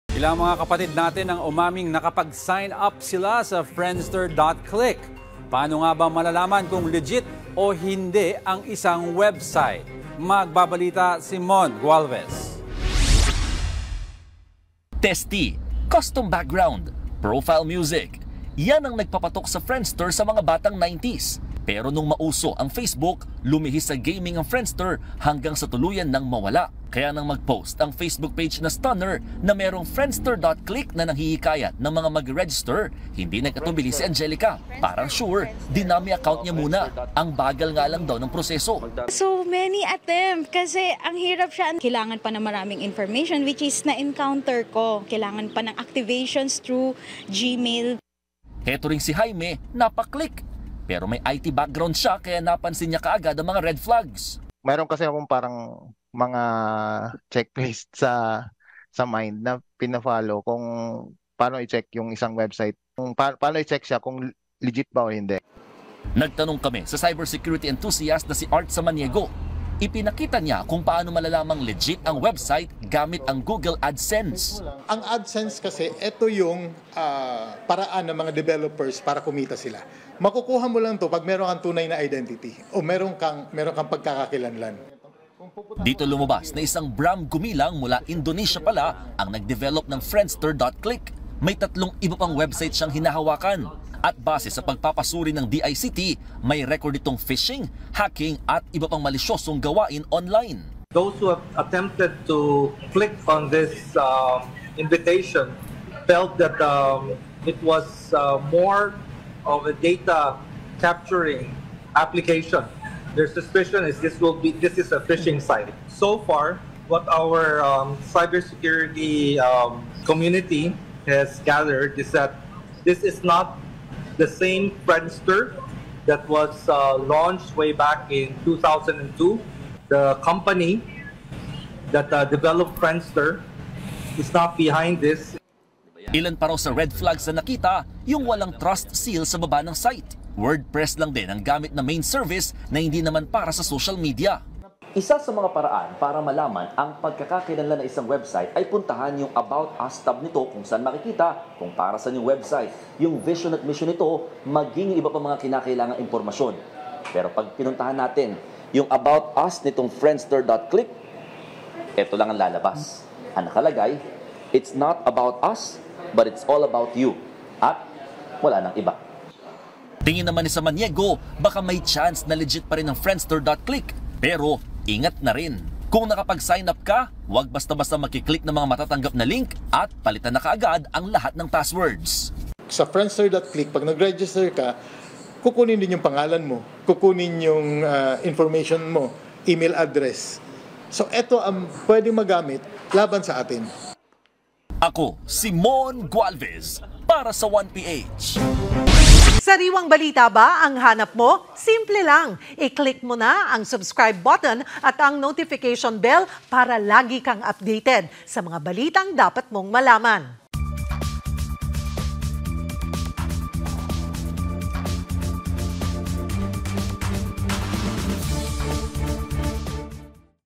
Publiko, pinag-iingat sa pagreregister sa kahina-hinalang Friendster website
News Balita LiveNewsTodayPhilippinesOnePH Pinag-iingat ang publiko sa pag-register sa Friendster.click website dahil sa kahina-hinalang mga feature nito! | via Mon Gualvez